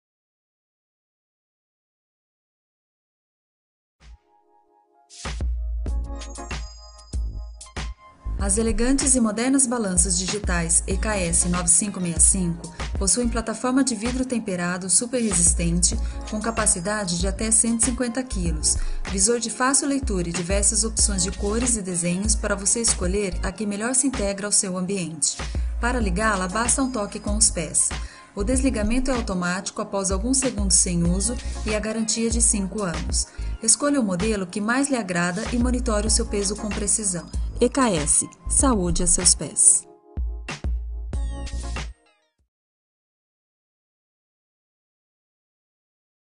Balança Digital EKS 9565 PO
Com a Balança Digital EKS 9565 PO você vai ficar sempre de olho no seu peso e cuidar da sua saúde diariamente.
Leve e resistente, ela possui alta sensibilidade, plataforma de vidro e visor de fácil visualização com dígito extragrande.
Pesa em quilos, libras e stones. Com design slim, ela é ideal para uso doméstico e suporta até 150 quilos.
Para acioná-la basta um simples toque com pressão na plataforma. Como ela tem um visual supermoderno combina com qualquer ambiente.